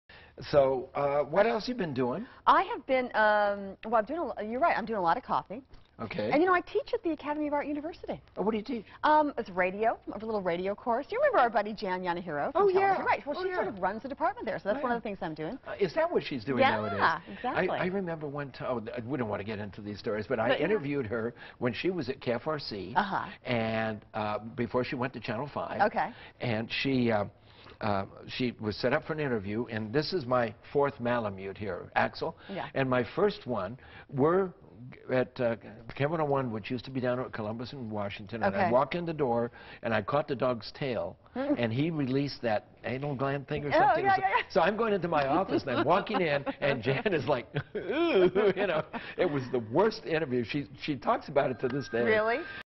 James Gabbert on Jan Yanehiro
KOFY TV 20 in San Francisco during a marathon of the series Hazel. Along with James is radio personality Celeste Perry.

If you would like to see more videos please consider making a donation